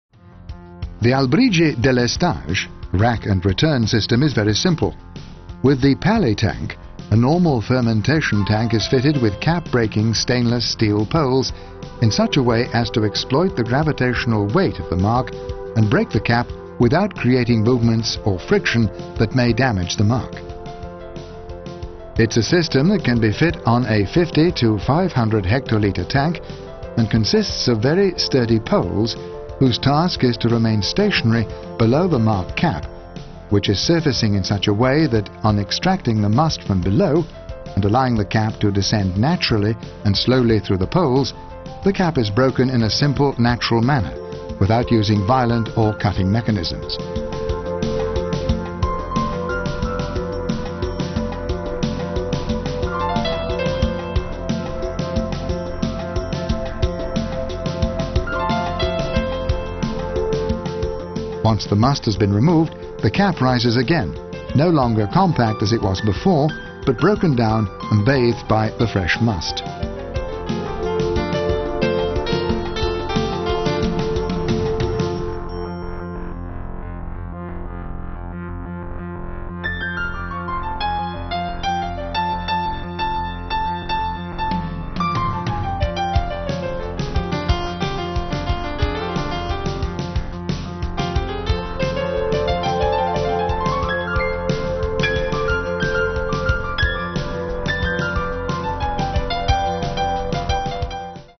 Delestage - Palitank ∙ Albrigi
In Albrigi's Palitank the classical wine-making process known as deléstage is offered in a totally automated plant. In the deléstage all the must is taken from the lower part of the fermenting tank and from there, by gravity, is sent to a collecting tank placed at a lower level. Then the must is pumped to the fermentors again and spread over the grapes skin cap by means of a rotating head.